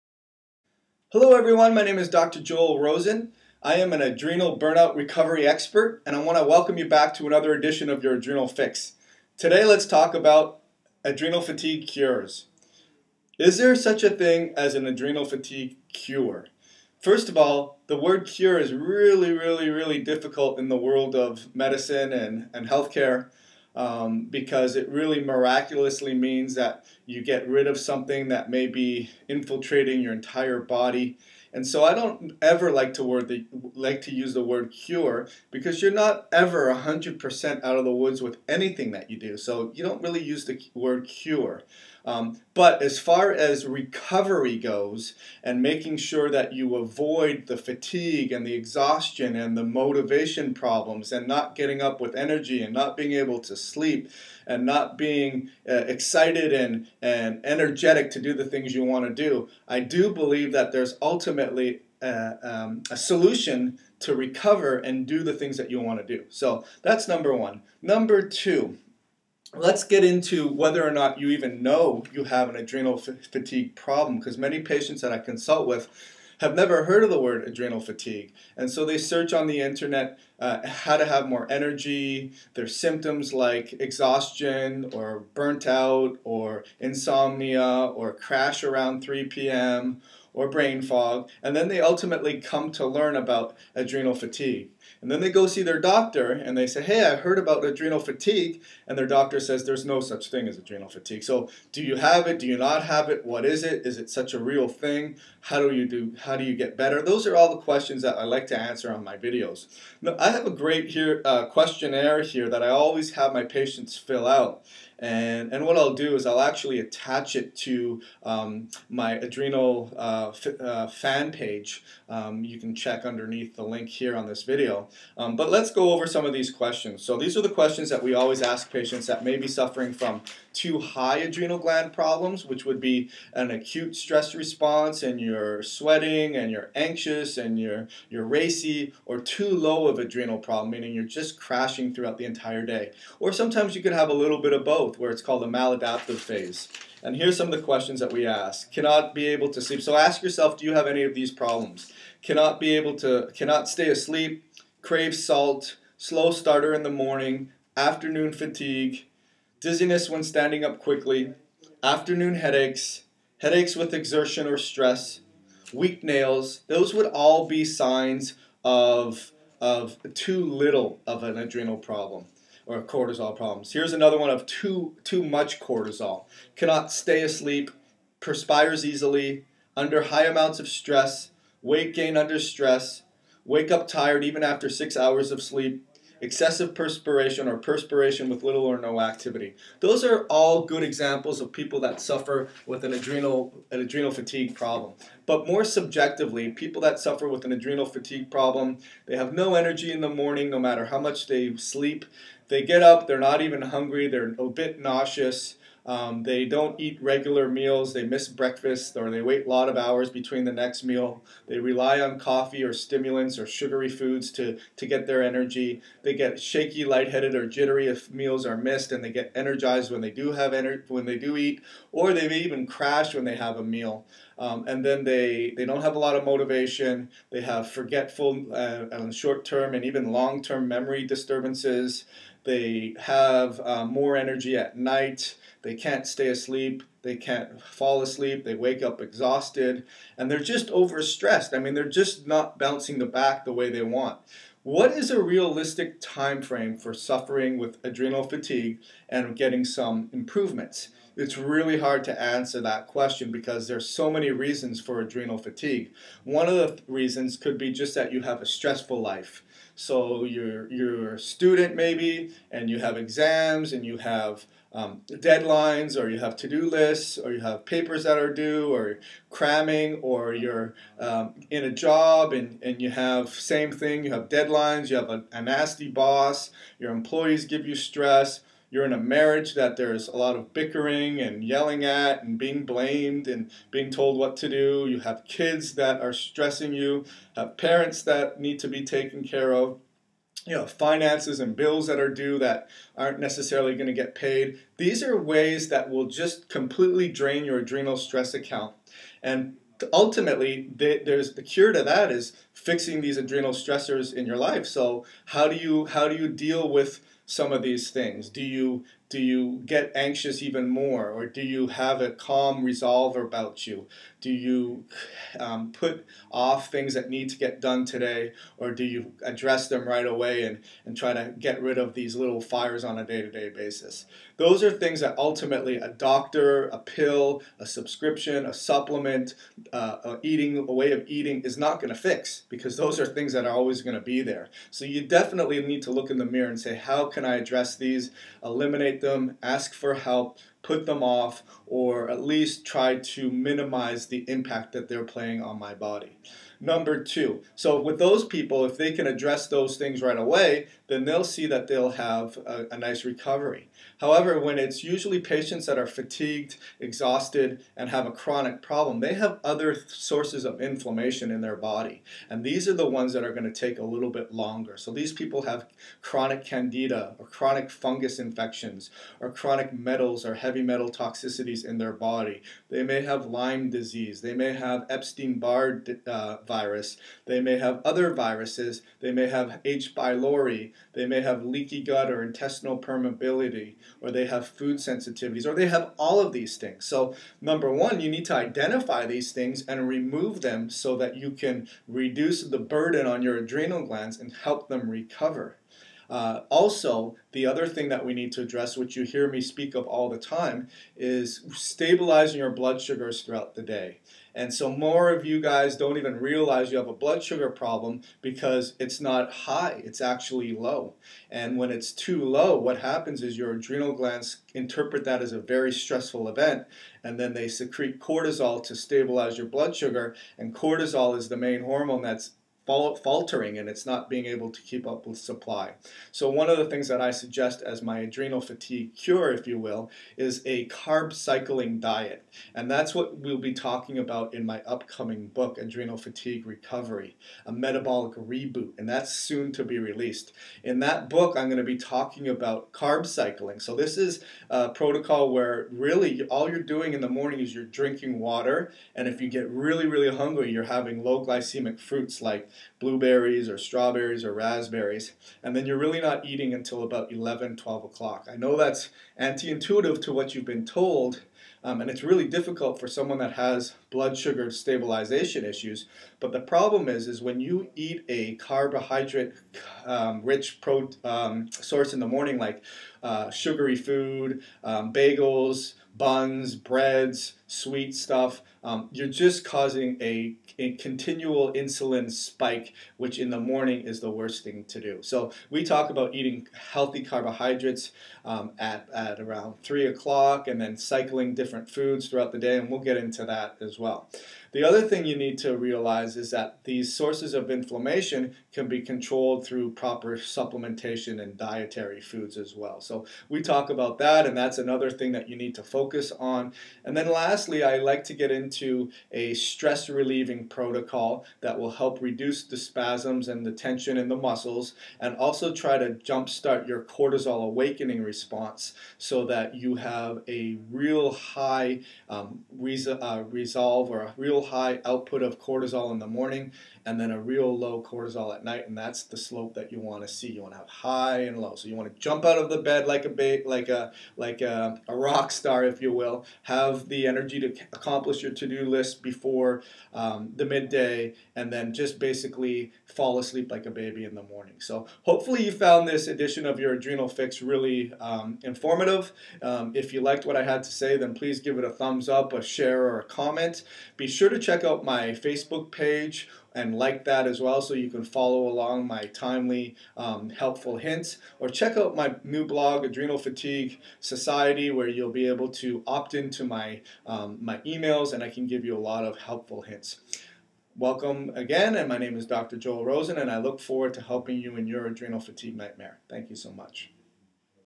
Adrenal Fatigue Cure
Is there such a thing as a "cure" to adrenal fatigue? Learn how to go about the best way to recovery from adrenal fatigue.